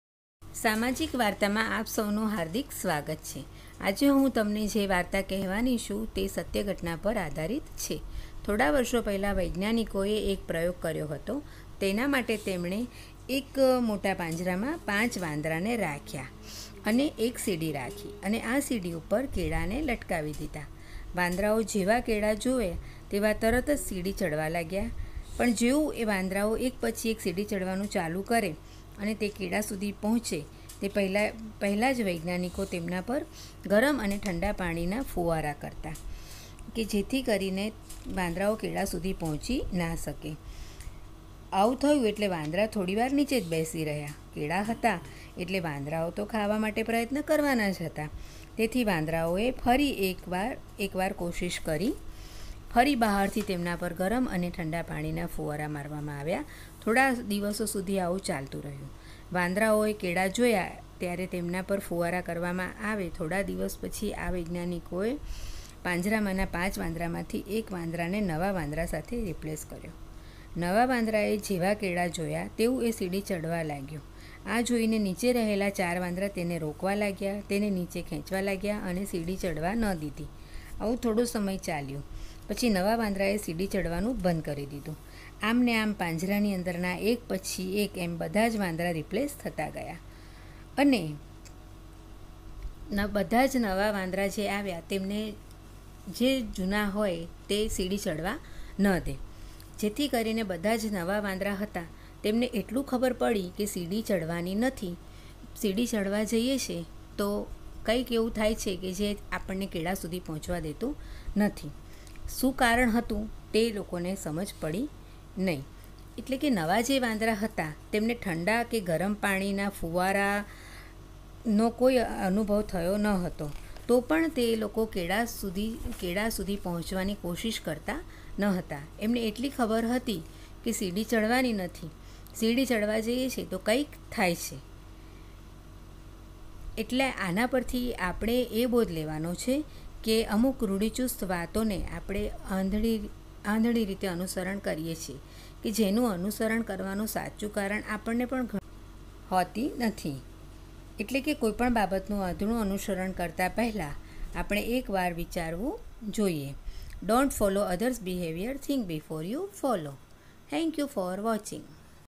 શુતમે અનુસરો તે પહેલા વિચારો છો?
એક સત્ય ઘટના પર આધારિત આ વાર્તા છે
Dont follow others behavior
Think before you follow